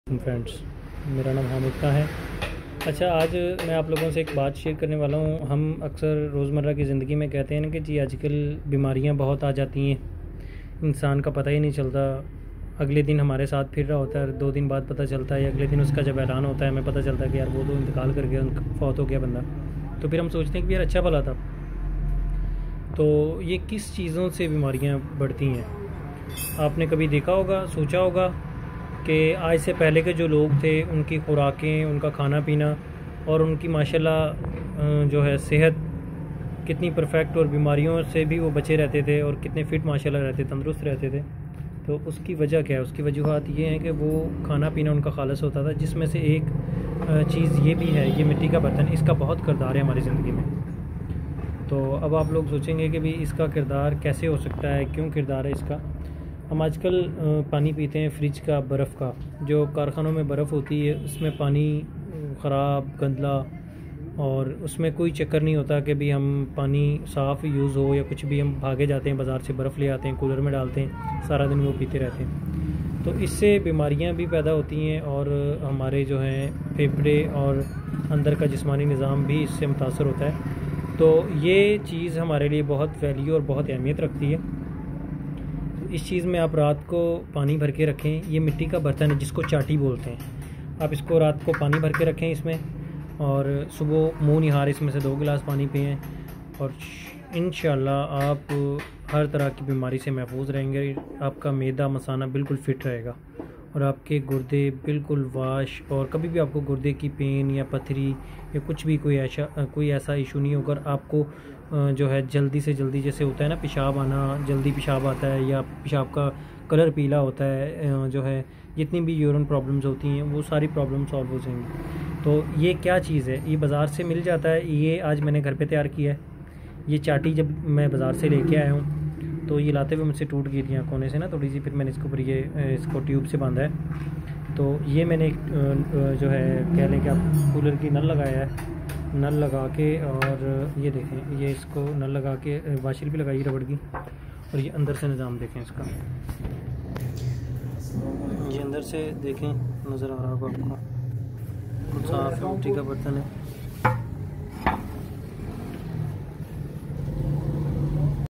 70 Bemarion ka elaj
mitti k bartan  main pani Peena 70 kisam ki beemarion se bchata hy